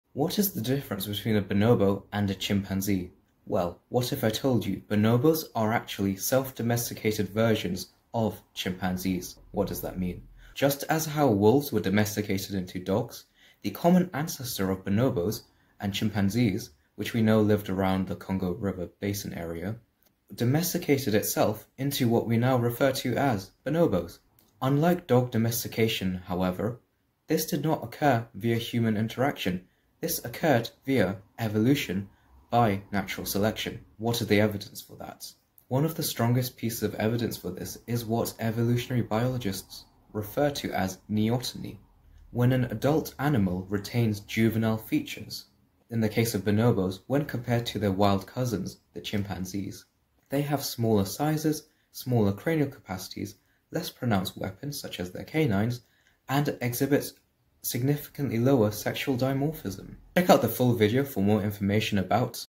The unique evolutionary history of the bonobo!
What's the difference between a Bonobo and a Chimpanzee? 🤔 Discover how Bonobos are self-domesticated versions of Chimpanzees! Learn what self-domestication means and how evolution by natural selection led to bonobos retaining juvenile features like smaller size, cranial capacity, and reduced canines – a phenomenon known as Neoteny.